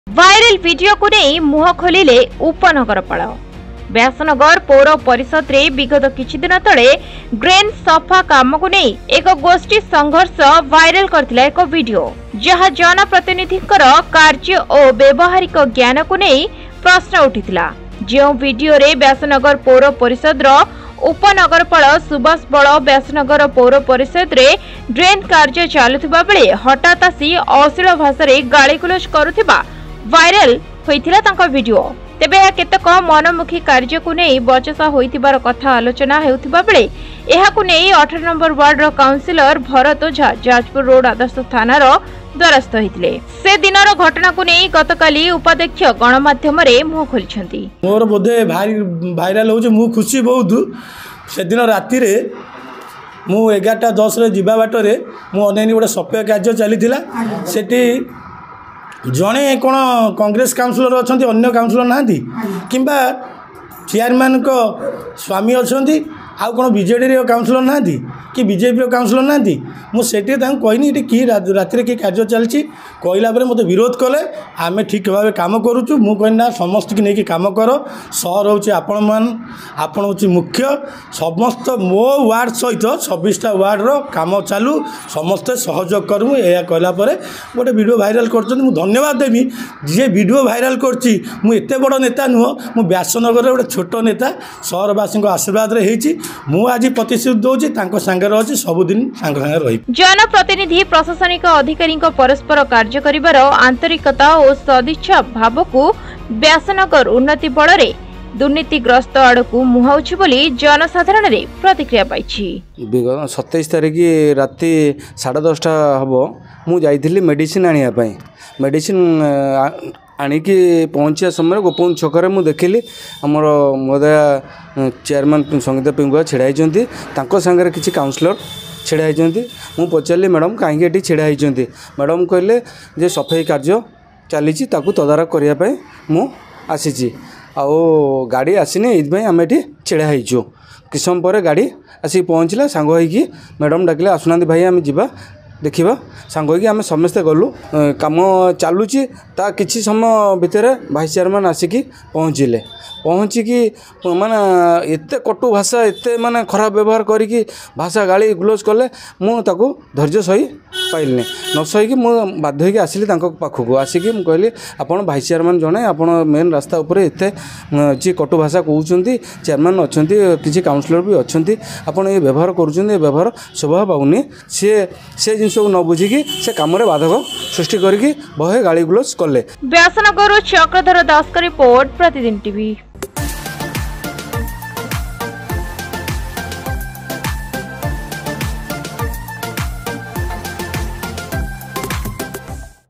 ଡ୍ରେନ ସଫା କୁ କେନ୍ଦ୍ର କରି ହୋଇଥିବା ଗୋଷ୍ଠି ସଂଘର୍ଷ କୁ ନେଇ ମୁହଁ ଖୋଲିଲେ ଉପନଗରପାଳ l PratidinTv
ଡ୍ରେନ ସଫା କୁ କେନ୍ଦ୍ର କରି ହୋଇଥିବା ଗୋଷ୍ଠି ସଂଘର୍ଷ କୁ ନେଇ ମୁହଁ ଖୋଲିଲେ ଉପନଗରପାଳ l